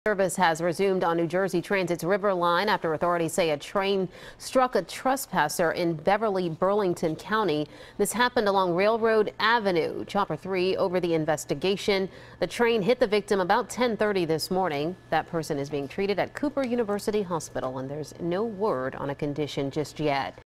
Train Hits Trespasser In Burlington County
Service has resumed on New Jersey Transit's River Line after authorities say a train struck a trespasser in Beverly, Burlington County.